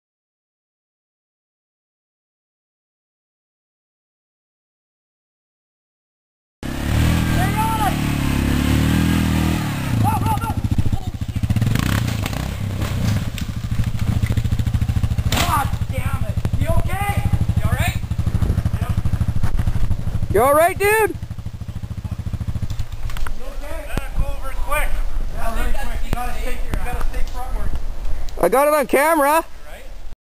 BigD74's Roll at Tamahi
BigD74 rolls his Yamaha Grizzly end-over-end down a steep trail in the Tamahi area near Chilliwack, BC, Canada.